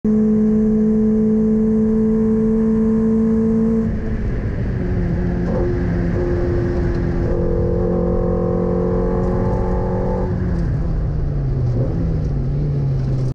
dandy road, lotus elise
getting better over dandy road as the day went on